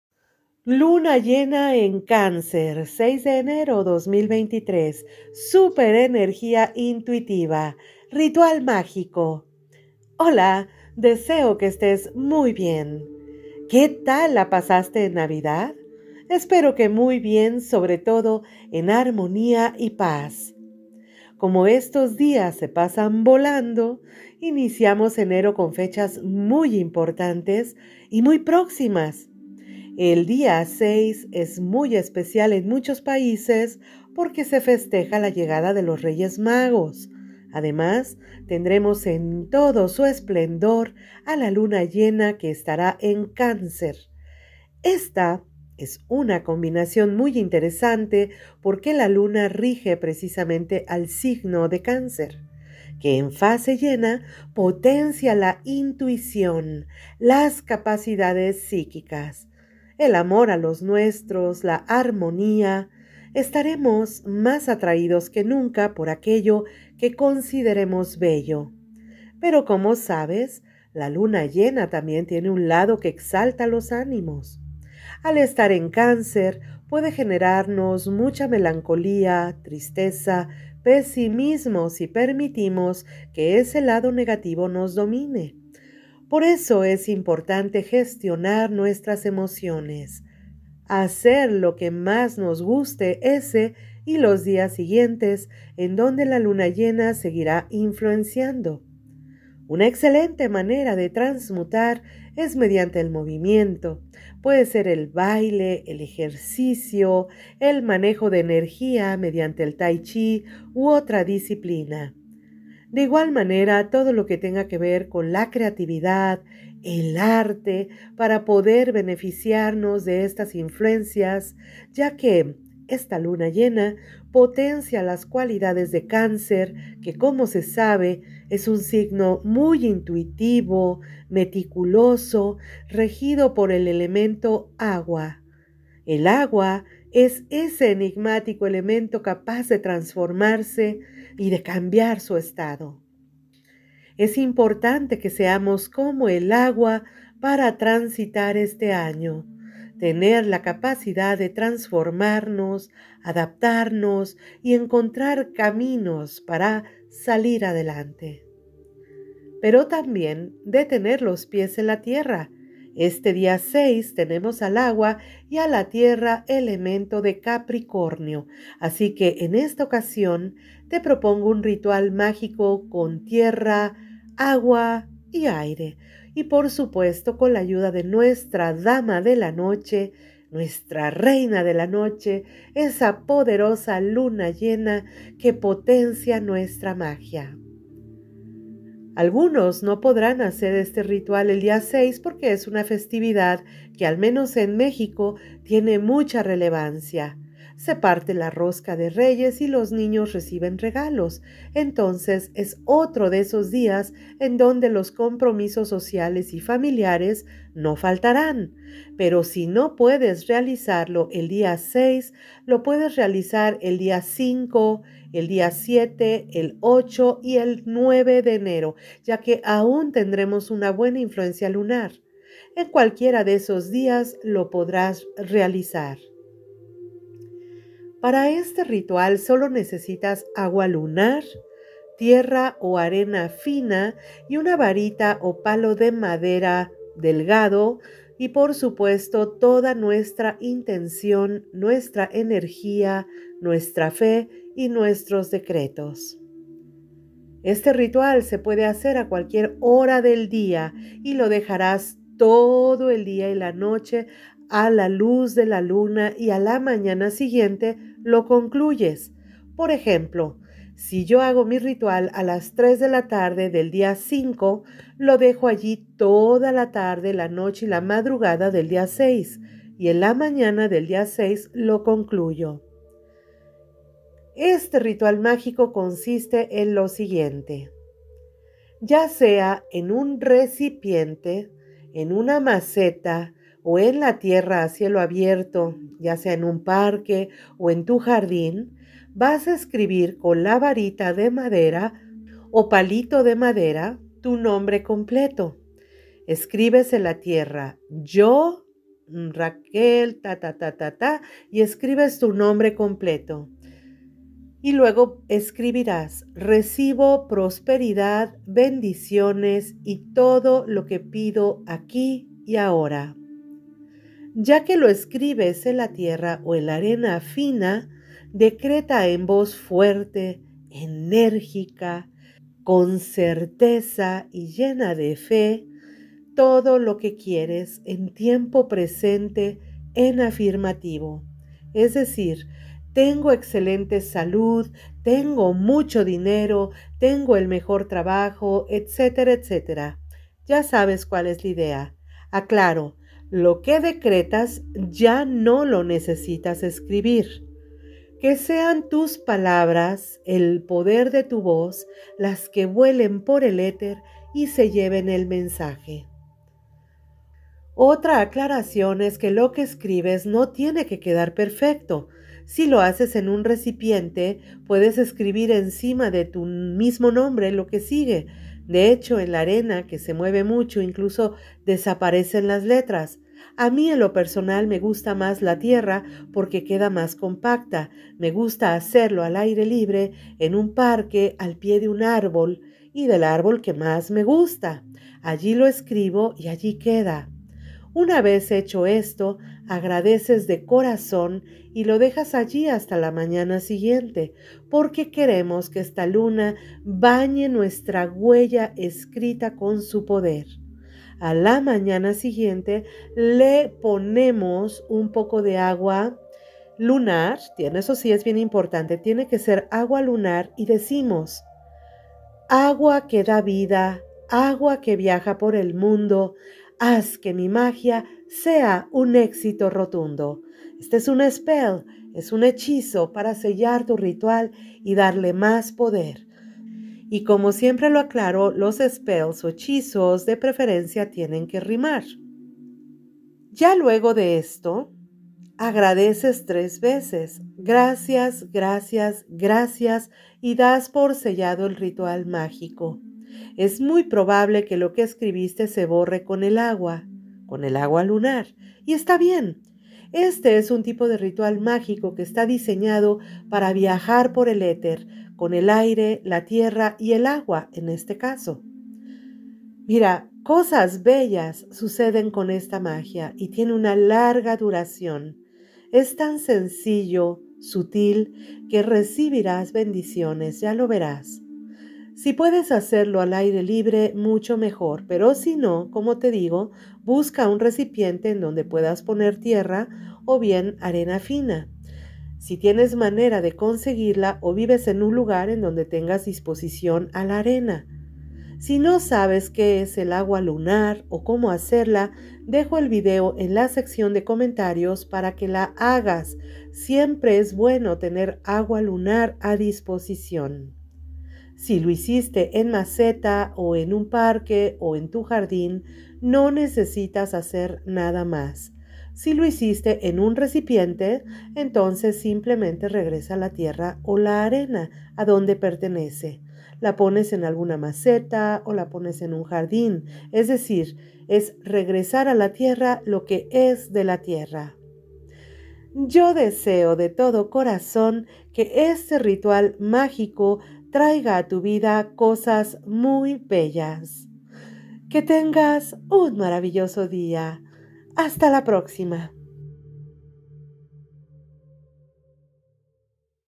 LUNA LLENA EN CÁNCER 6 ENERO 2023 RITUAL MÁGICO Astrología
Luna llena en Cáncer 6 Enero 2023 Ritual mágico para atraer cosas bellas a tu vida. Prosperidad, riqueza, salud, amor, trabajo. Astrología, numerología.
Agua Lunar. Cómo hacerla